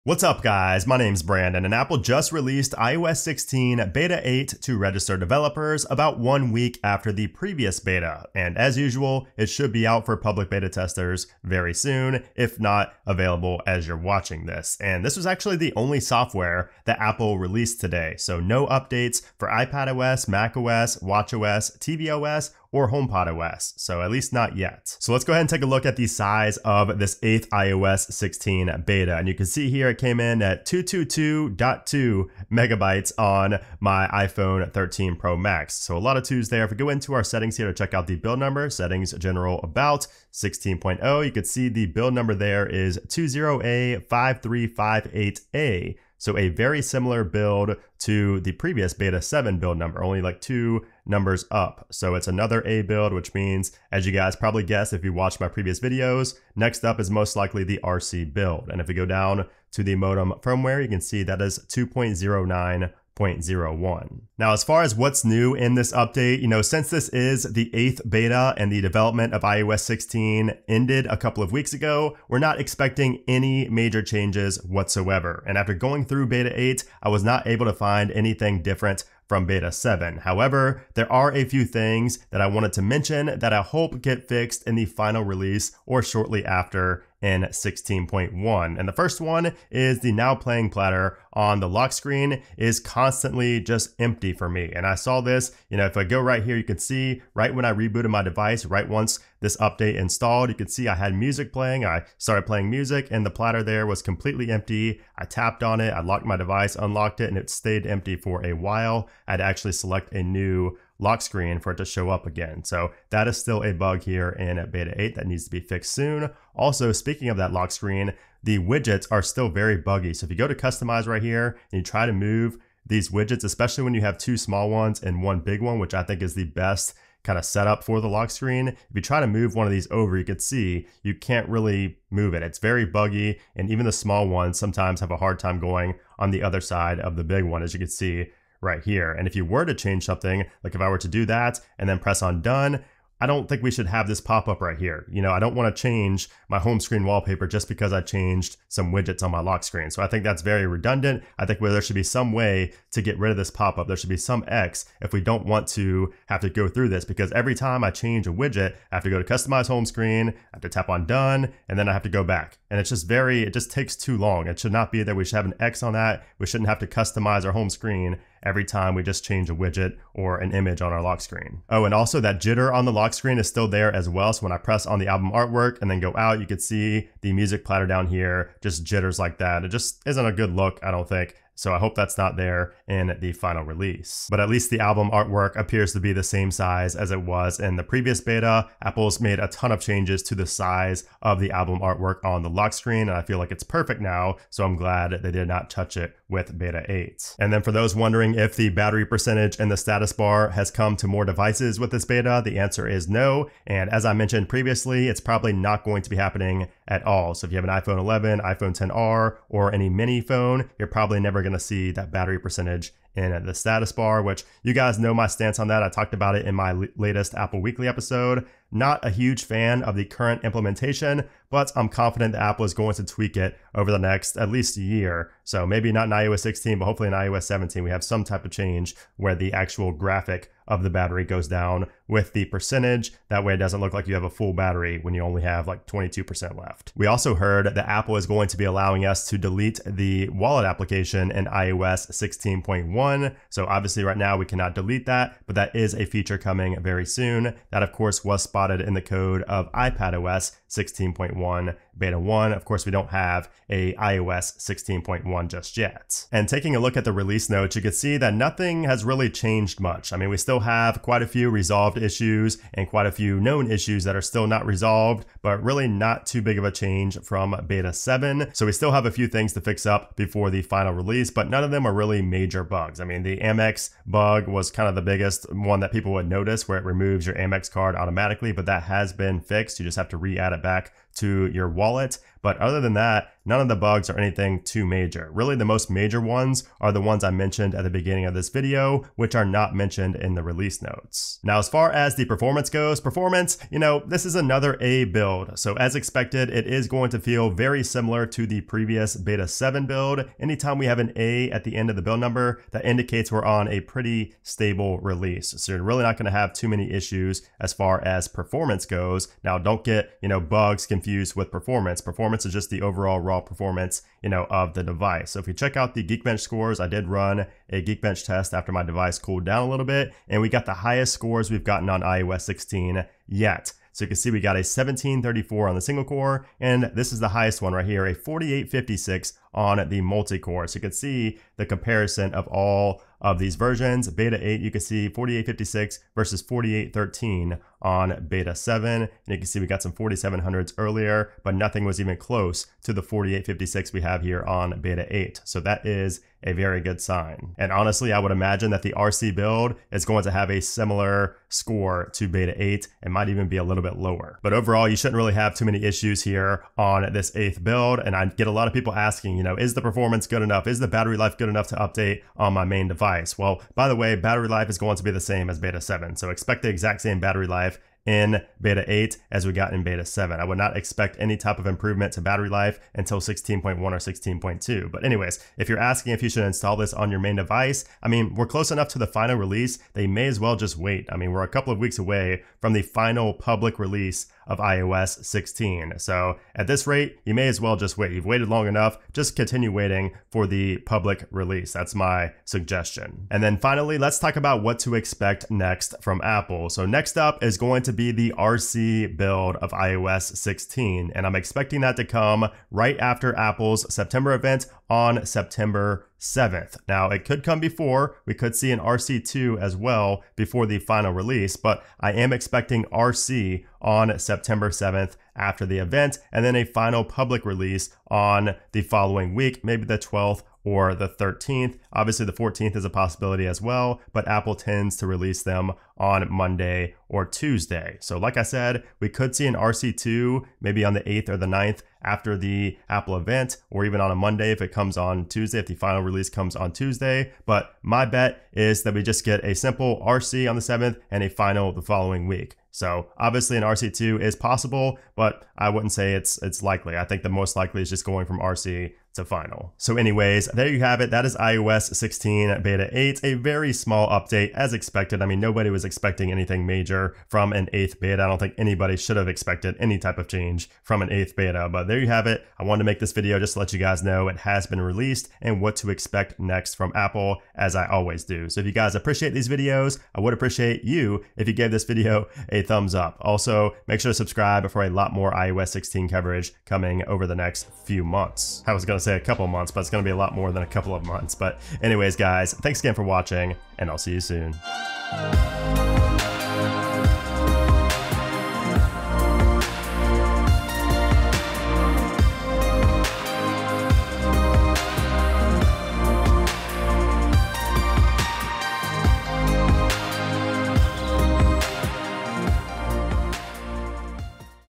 iOS 16 Beta 8 Released - What's New?
iOS 16 Beta 8 Released - What's New? | iOS 16 Features & Changes

Apple just released iOS 16 Beta 8 to registered developers about 1 week after the previous Beta 7. But surprisingly, Apple did not release any new updates for iPadOS, watchOS, tvOS 16, and HomePodOS 16.

But as always, in this video, we're going to discuss what's new in the iOS 16 Beta 7 software, including new features and changes, bugs, bug fixes, performance, battery life & when to expect iOS 16 Beta 8 and iOS 16 RC/Final!

Chapters:
0:00 Everything Released
0:25 Size, Build Number
1:04 What’s New & What’s Wrong
3:17 Battery Percentage
3:56 Wallet
4:16 Release Notes
4:51 Performance
5:54 Should You Update Now?
6:33 iOS 16 RC & Final Release Date
7:29 Conclusion

What do you think of iOS 16 Beta 8 so far? Are you excited for the final version?

🎵Outro:

▬ STAY UP-TO-DATE! ▬